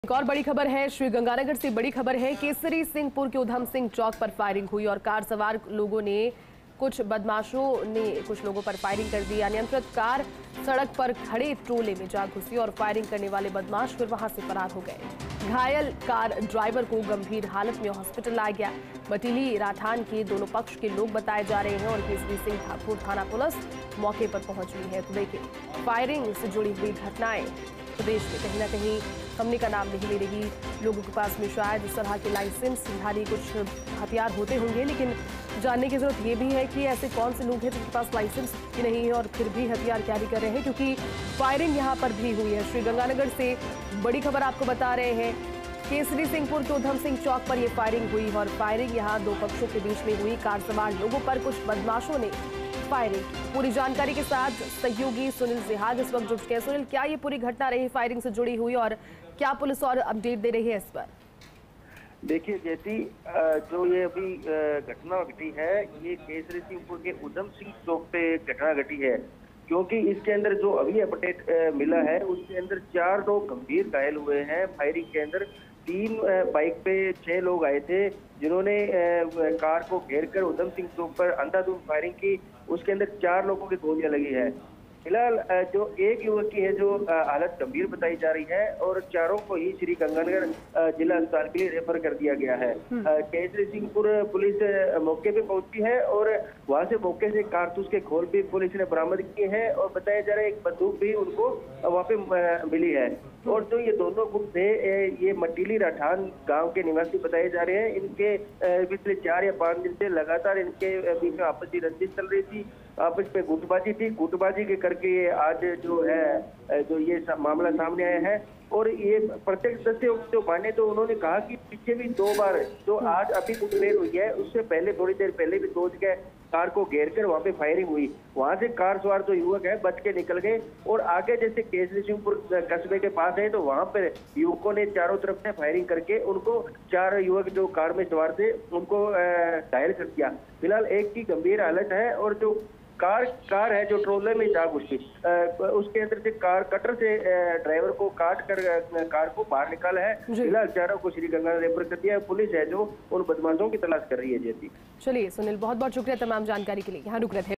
Sri Ganganagar में कार सवार युवकों पर अंधाधुंध फायरिंग, 4 युवक हुए घायल
About this Video:
श्रीगंगानगर से इस वक्त की बड़ी खबर ,कार सवार युवकों पर अंधाधुंध फायरिंग,फायरिंग में गोली लगने से कार सवार 4 युवक हुए घायल,घायल युवकों में से एक युवक की हालत गंभीर
सभी घायलों को श्रीगंगानगर किया रेफर,केसरीसिंहपुर के उधम सिंह चौक पर हुई फायरिंग
3 बाइक पर आए कई युवकों ने की अंधाधुंध फायरिंग,पुलिस ने मौके से कारतूस के खोल सहित एक हथियार किया बरामद,अचानक फायरिंग होने से कार अनियंत्रित होकर ट्रोले में घुसी,मटीली राठान के बताए जा रहे दोनों तरफ के लोग, केसरीसिंहपुर थाना पुलिस पहुंची मौके पर